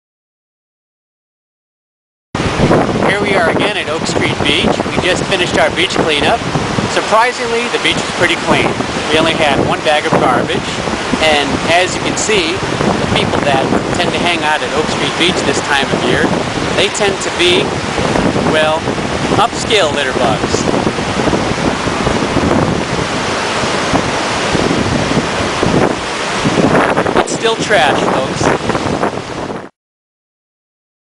Open Water Chicago - April Adopt-a-Beach at Oak Street Beach
I do occasional volunteer work for the Alliance for the Great Lakes. Since were always out at Oak Street Beach, I chose this one for the Adopt-a-Beach program. The beach is not too populated this time of year so the litter load is minimal. But as you can see, the Gold Coast cappuccino crowd sure needs to clean up its act!